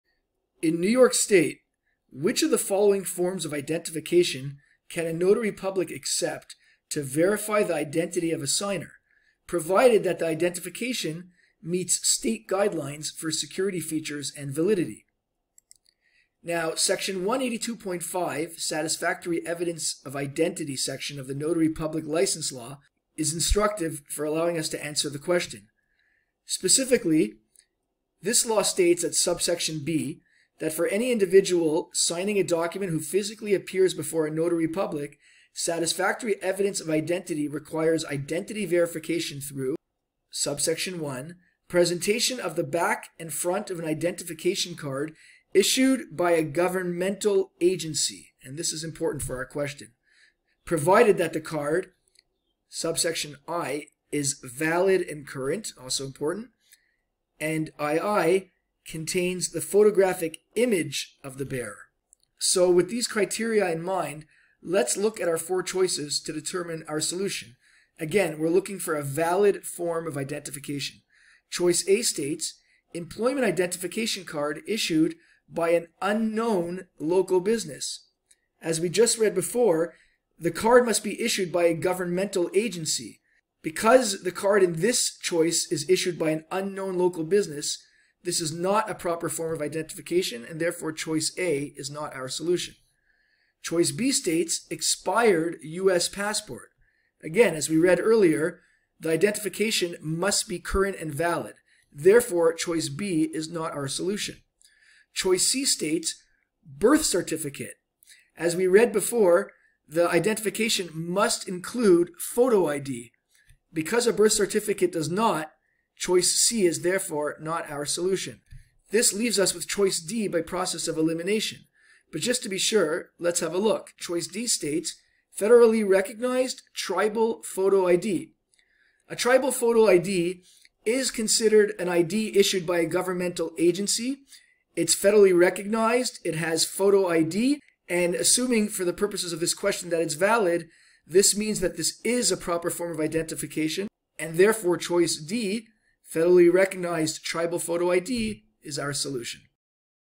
NYS Notary Public iPREP Assessment Test Q24
A solution to one of our NYS Notary Public Exam practice questions. Explore iPREP's NYS Notary Public Exam prep course